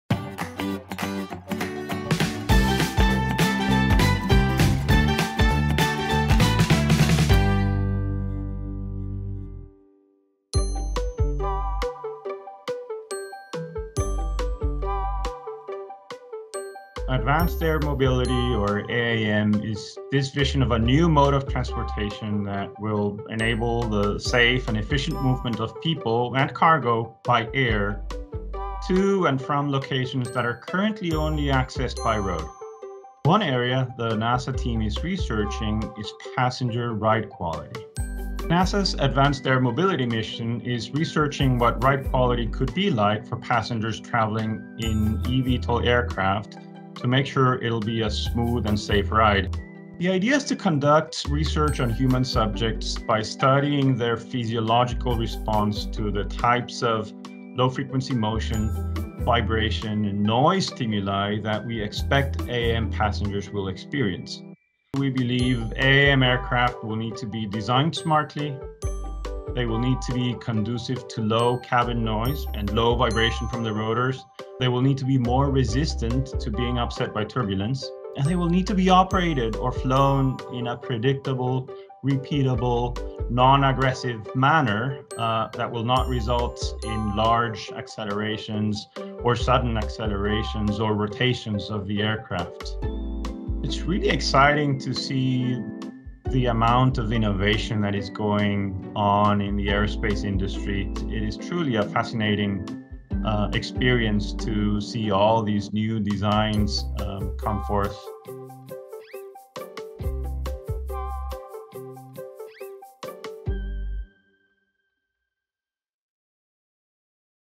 NASA's Advanced Air Mobility Playbook: Ride Quality
NASA’s vision for Advanced Air Mobility, or AAM, is to map out a safe, accessible, and affordable new air transportation system alongside industry partners, community partners and the FAA. NASA is researching what ride quality could be like for passengers traveling in eVTOL aircraft, to make sure it is a smooth and safe ride. In this episode of NASA's Advanced Air Mobility Playbook, RVLT Project Flight Dynamics and Control Technical Lead Carlos Malpica explains why NASA is uniquely qualified for this research and why it's important to the future of flight.

[ The animations shown in this video are artist concept animations of AAM and not technical renderings ]